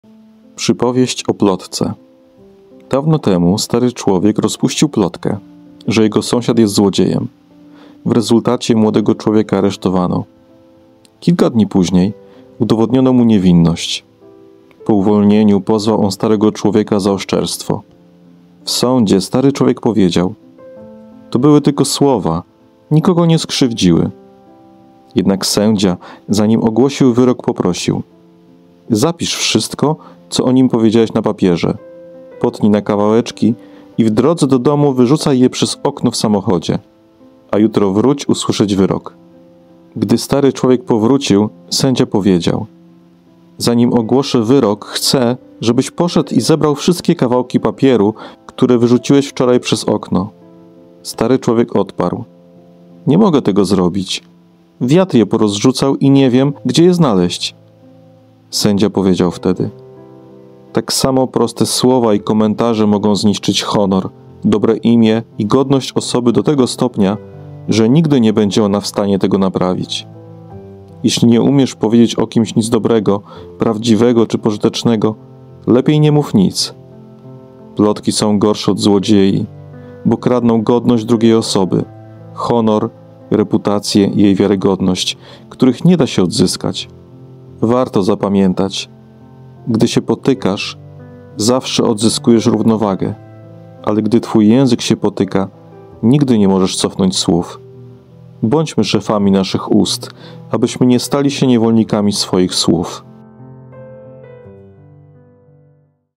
Przypowieść o plotce!
"Jeśli nie umiesz powiedzieć o kimś nic dobrego, prawdziwego, czy pożytecznego, lepiej nie mów nic."

Oprócz tego książki a także bajki dla dzieci w formacie audiobooka i e-booka można pobrać w abonamencie i kupić w Legimi oraz Empik Go.